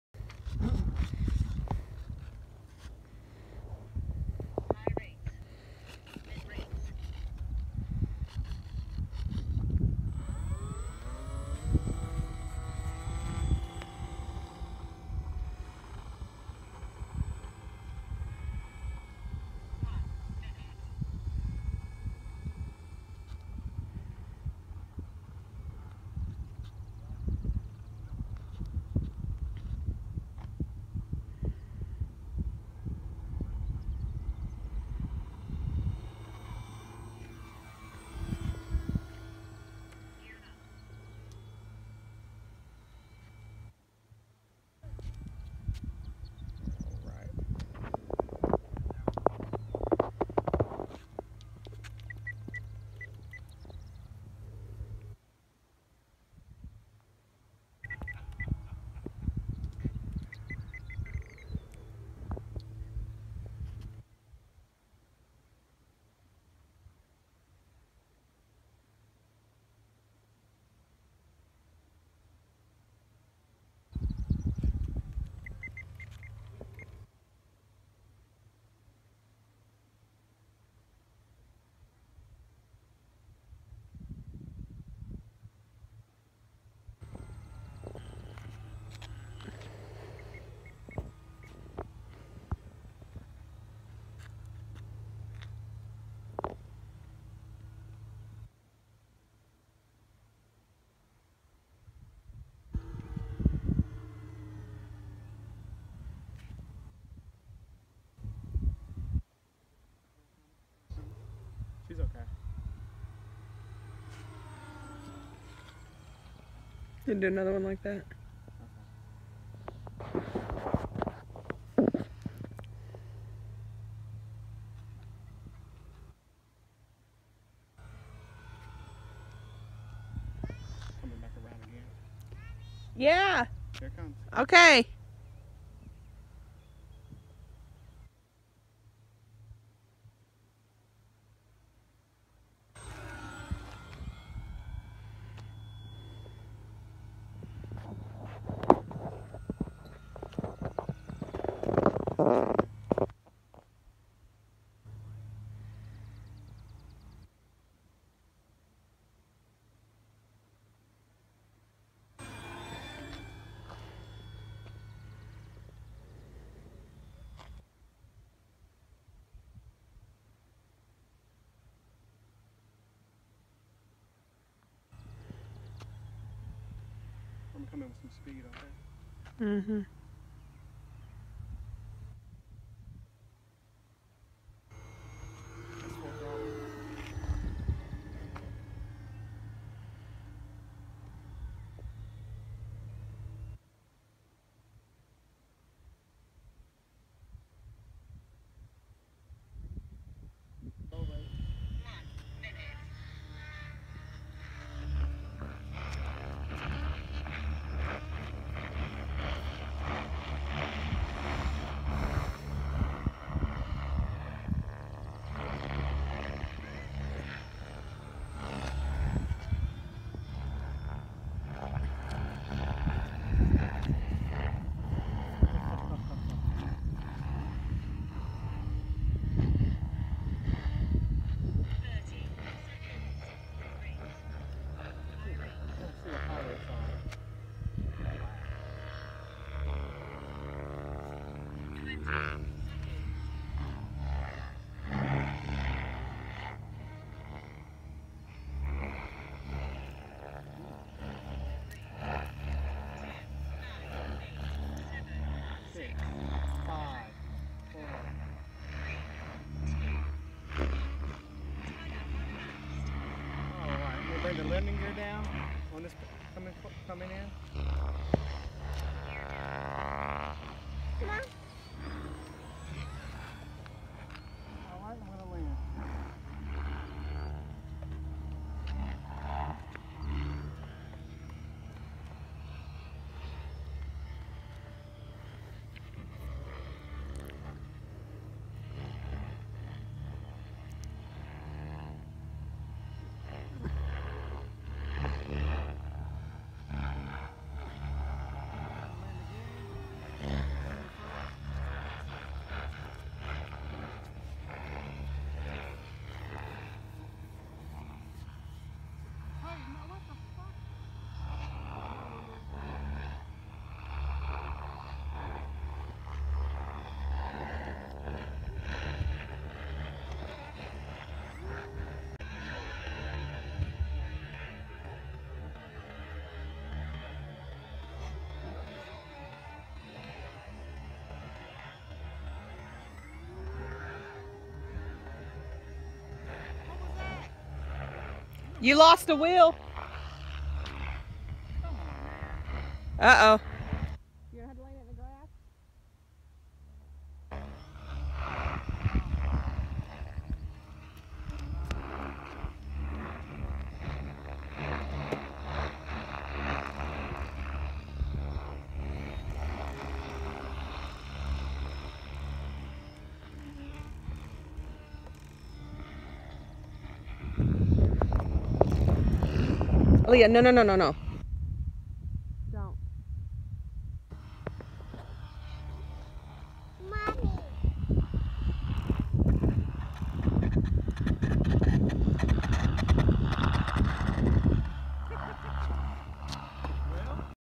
Durafly T28 1100mm Maiden
We had a great opportunity to get away from the city which I love to do and go to the flying field at the AMA headquarters in Muncie Indiana. My wife did an amazing job I think taking video of my Maiden flight of the Durafly 1100mm T-28.  Enjoy I did lose the landing gear on  a landing attempt. However I was able to successfully lad it.